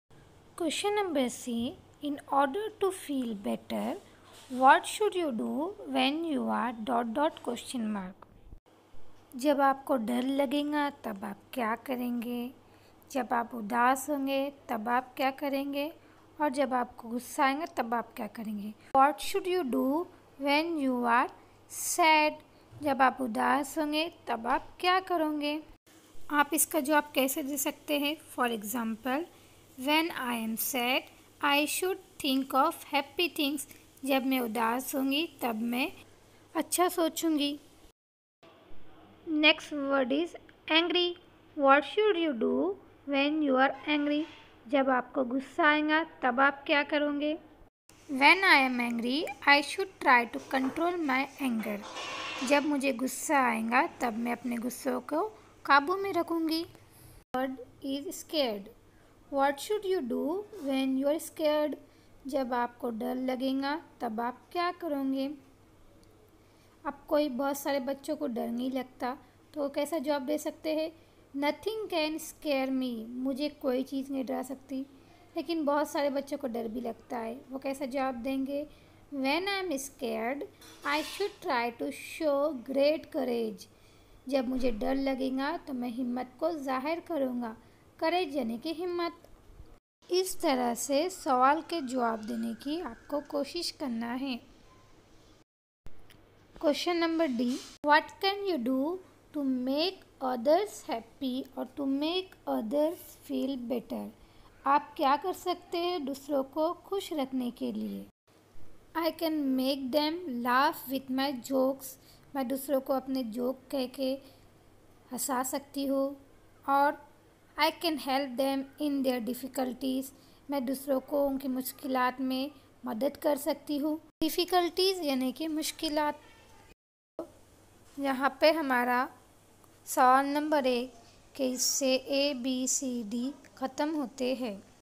1.2 Fun and Games with Tara and Friends
1.2 Fun and Games with Tara and Friends with urdu translation.. Good explanation.. With wods meanings. Question no 1 with example..